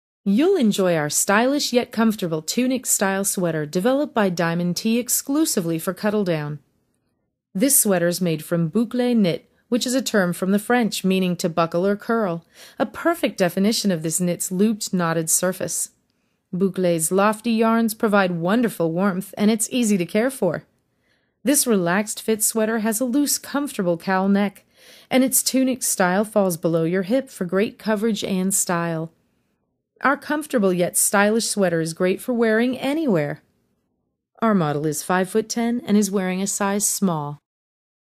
Cowl Neck Tunic Sweater in Soft Boucle Knit
Our stylish yet comfortable sweater is great for wearing day or night. Bouclé sweater knit is easy to care for. Bouclé is a French term meaning "to buckle or curl," a perfect definition of this knit's looped, knotted surface. Relaxed fit sweater has a loose cowl neck, and tunic style falls below hip. Made in Canada by Diamond Tea exclusively for Cuddledown.